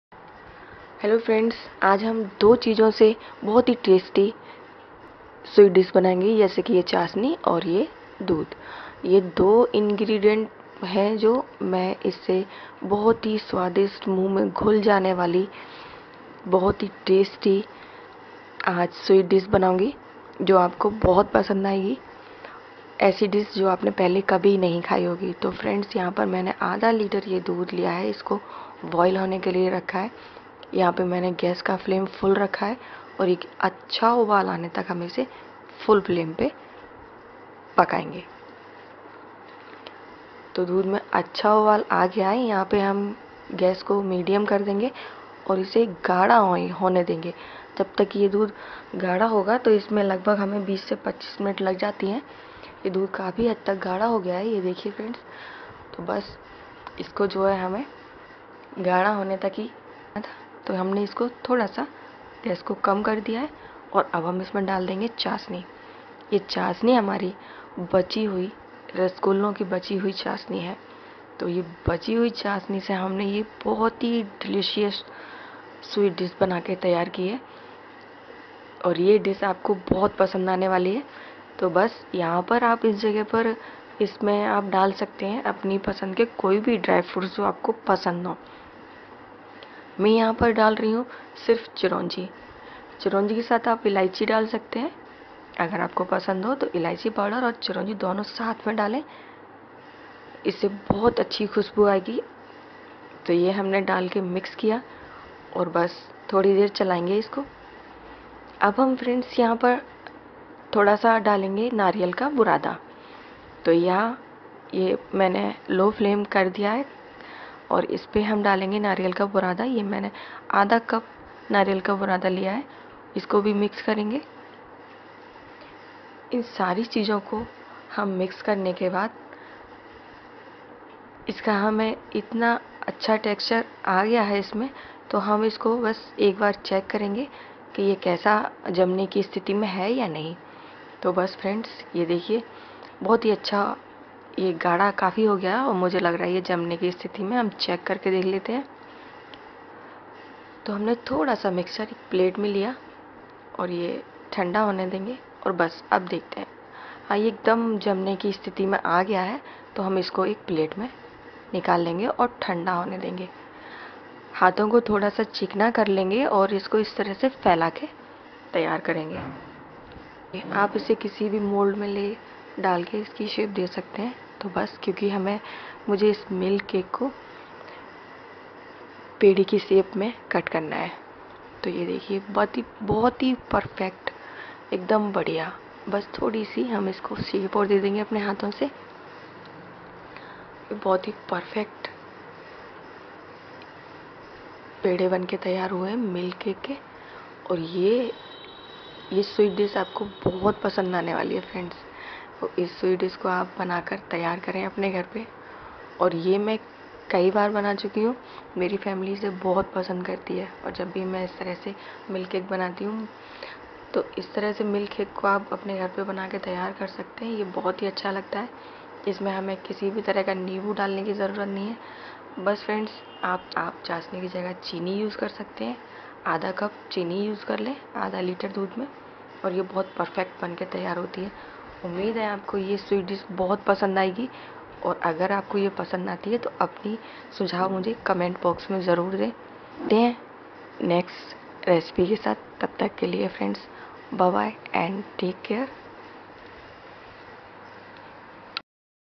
बची हुई चाशनी से बनी है स्वादिष्ट मिठाई \\ऐसी मिठाई ना आपने पहले देखी ना खाई \\ How To Make cake
मुख्य सामग्री   ***
  500 ग्राम दूध
इलायची पाउडर आधा चम्मच
चिरौंजी एक चम्मच
नारियल का बुरादा
चासनी
         मिल्क केक पेड़े बनाने की विधि ***
दूध को हाईफ्लैम पर गर्म करें उसे 20 से 25 मिनट तक गाढ़ा होने दें उसके बाद उसमें ड्राई फ्रूट्स डालें और चासनी डालकर गाढ़ा होने दें जब जमने की स्थिति में हो जाए तब एक प्लेट में ठंडा होने दे और कटर से अपनी मनपसंद आकार  दे  स्वादिष्ट पेड़े बनकर तैयार है